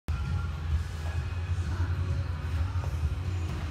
Targeting Hamstrings on a Skateboard: Effective Leg Workout
Take your hamstring workout to the next level with Hamstrings on a Skateboard! This unique exercise uses the instability of a skateboard to activate your hamstrings, glutes, and lower body in a way traditional exercises can’t. By incorporating the skateboard into your routine, you'll build strength, flexibility, and balance while working the muscles in your legs. Whether you’re an athlete or just looking for an effective lower body workout, this move is sure to challenge you.

What You’ll Get:

Step-by-step instructions for using the skateboard to target your hamstrings
Tips on form and technique to maximize results
A great way to improve flexibility, balance, and strength in your legs
A fun and challenging workout variation to add to your routine
Perfect for those looking to spice up their leg day or anyone looking to enhance their hamstring strength and mobility.

Share your progress in the comments below, and let’s sculpt our bodies together!